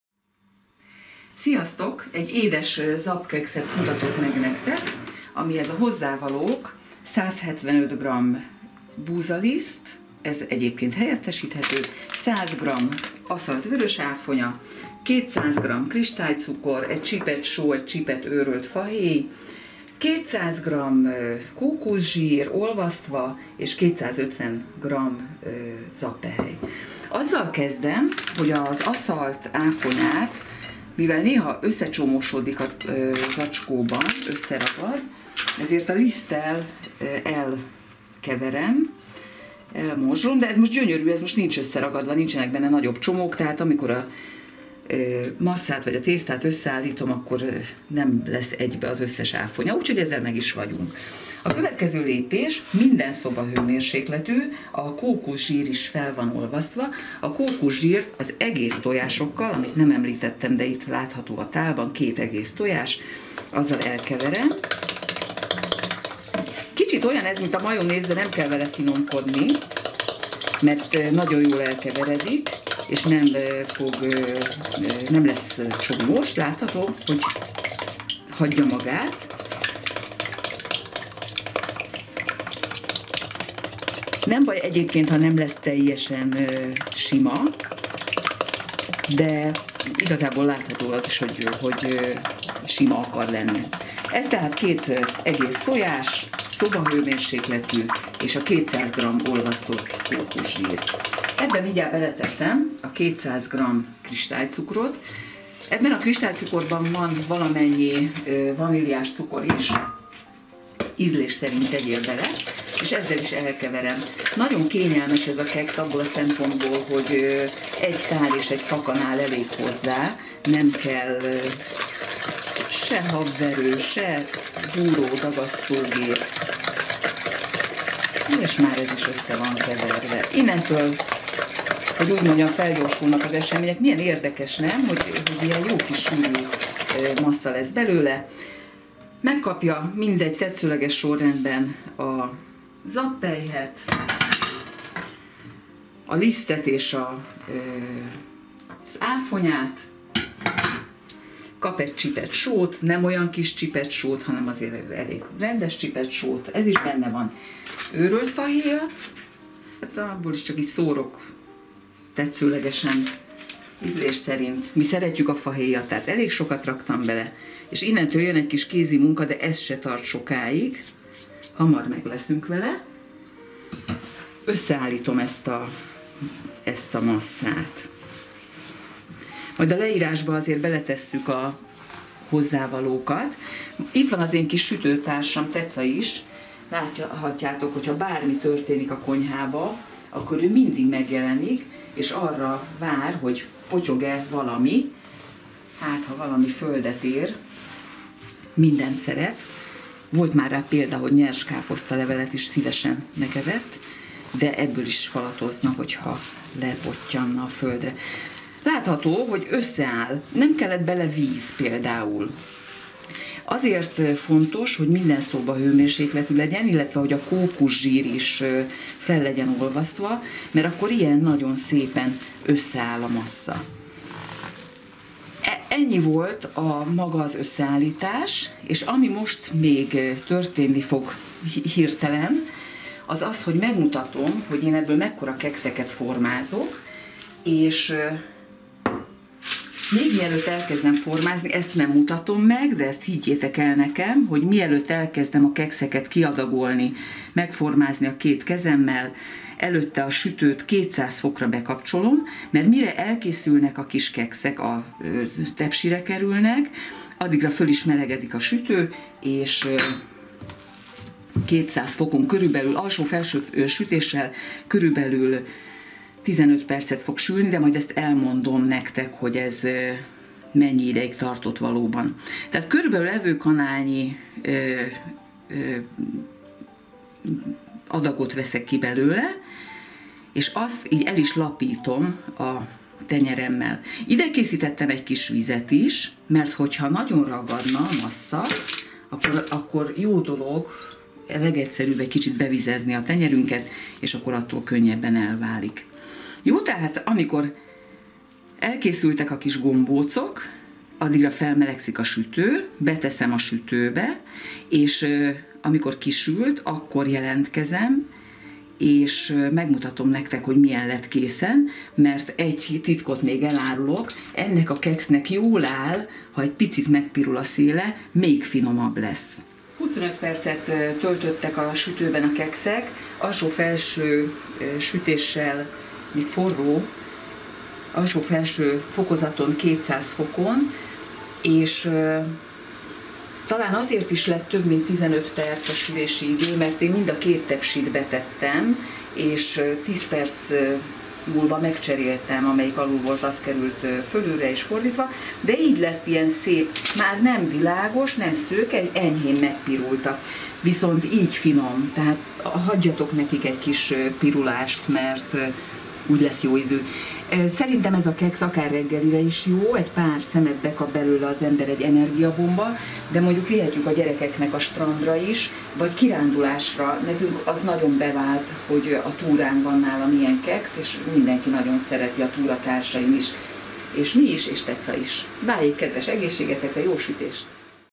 Egyszerű zabkeksz édes változat
Hozzávalók:
200 gr kókuszzsír
200 gr cukor
2 tojás
175 gr búza finomliszt
250 zabpehely
100 gr aszalt áfonya
őrölt fahéj
csipet só